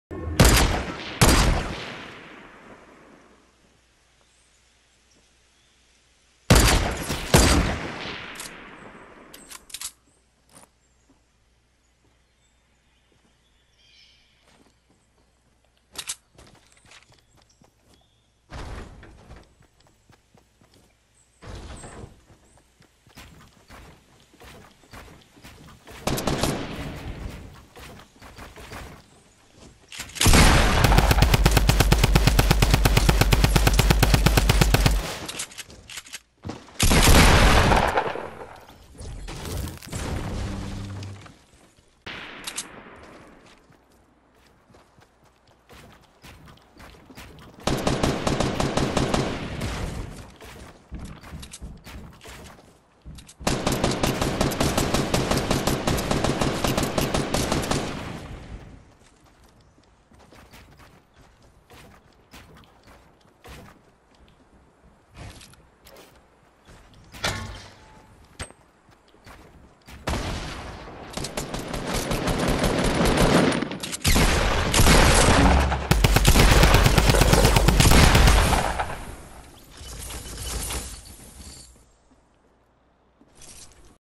FINALLY SWITCHED TO BUILDER PRO!!!
These were two kills from my second game on builder pro. first game I got fourth place, and got 2nd in this game. Still getting used to the controls, but thought it was about time I switched. Made these kills more effortless.